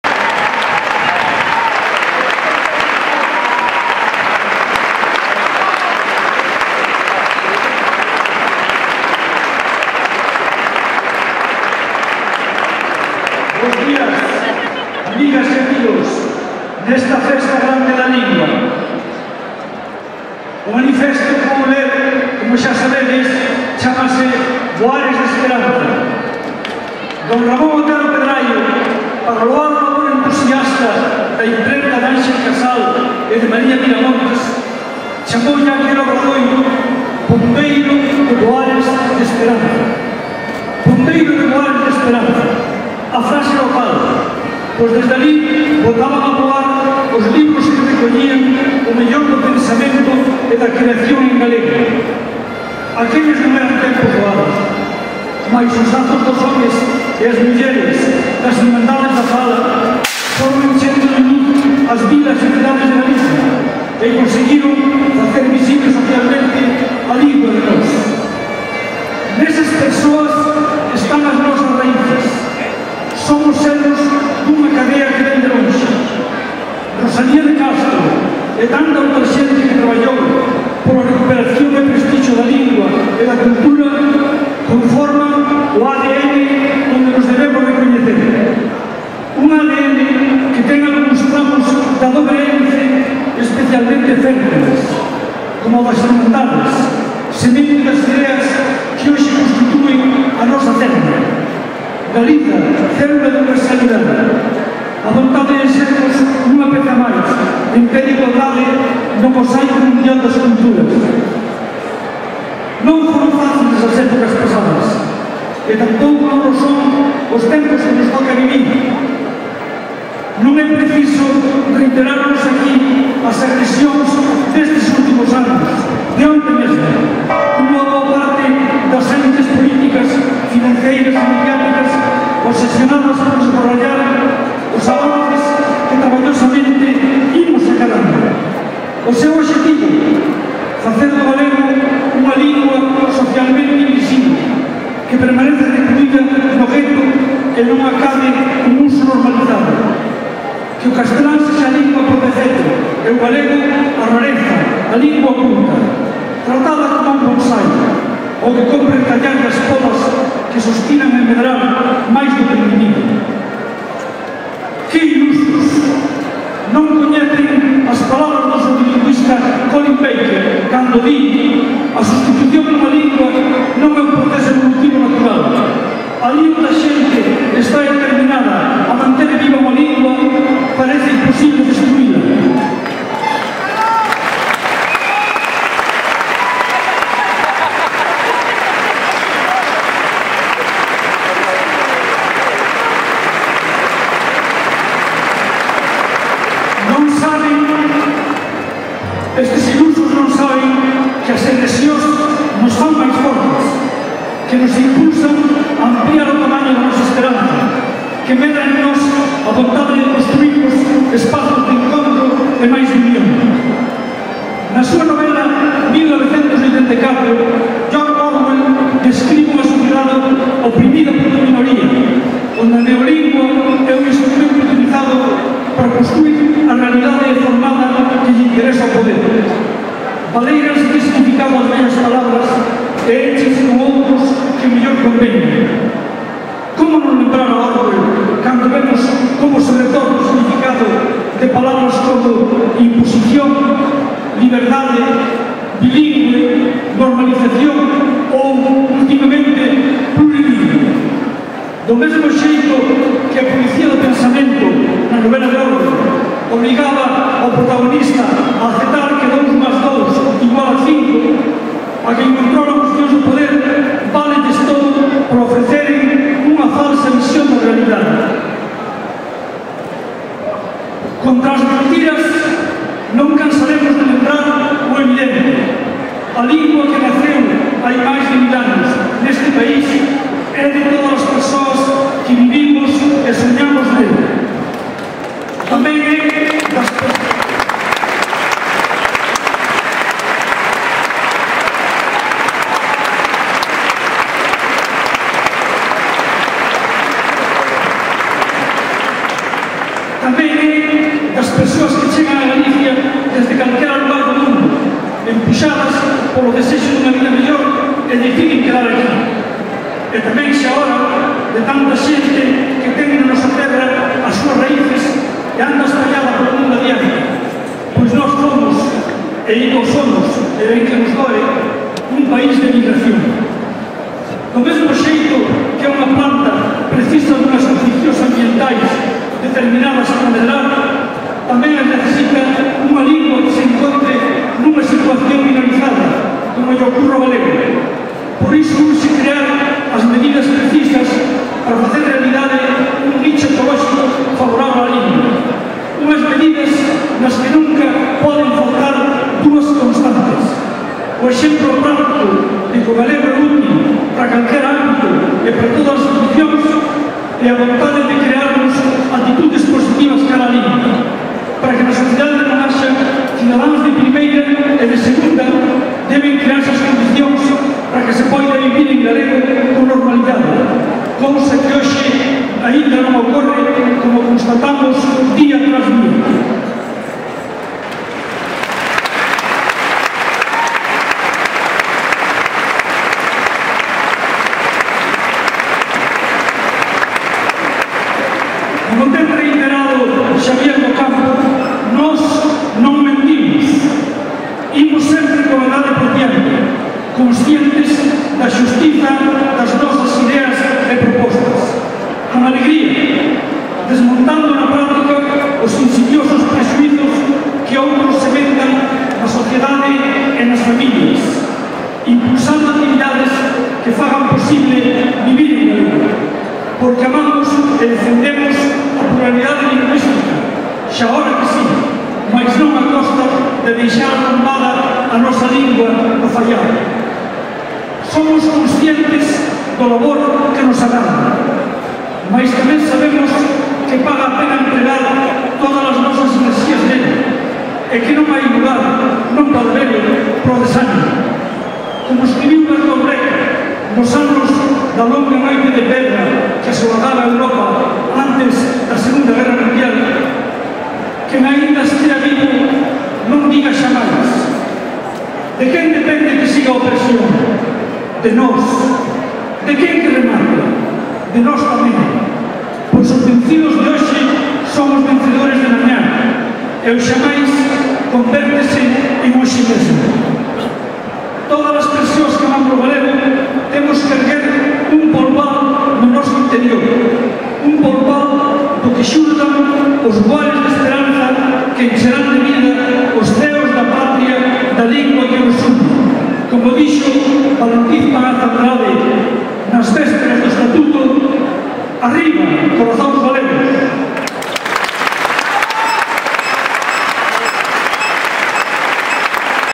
Dia das Letras 2012: Agustín Fernández Paz
Intervención na Quintana dentro da convocatoria de Queremos Galego.